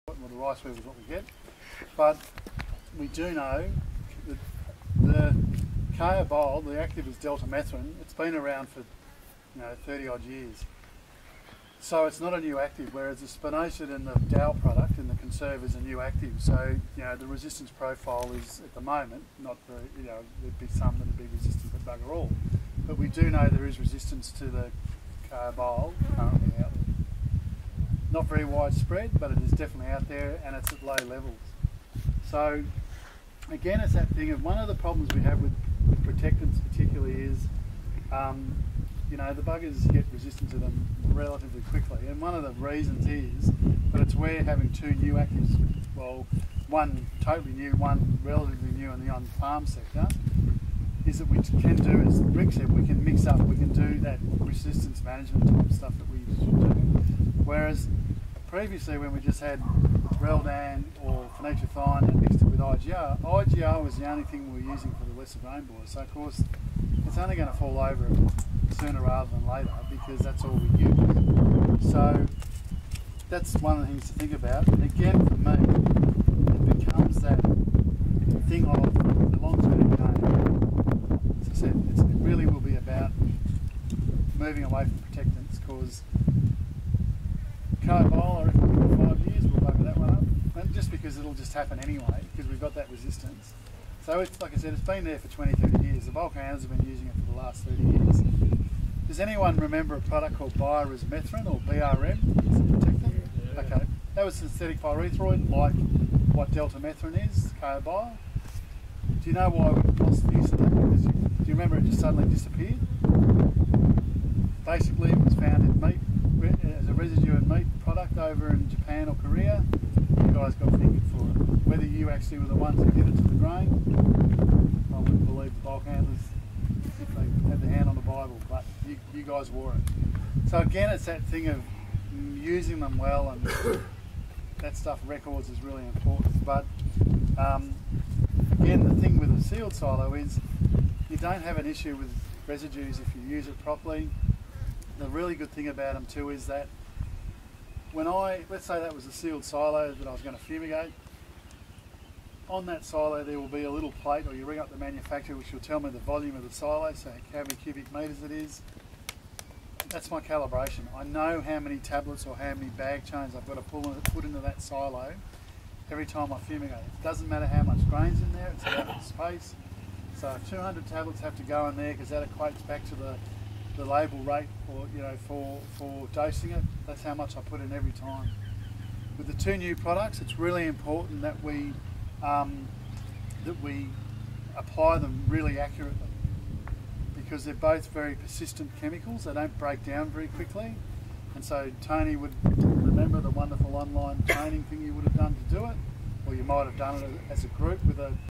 How to Use Grain Protectants Effectively
Peter Botta (PCB Consulting) explains the importance of using best practice when applying grain protectants, to minimise risk of residues, speed of breakdown and insect resistance. Video from Grain Storage Day 2017.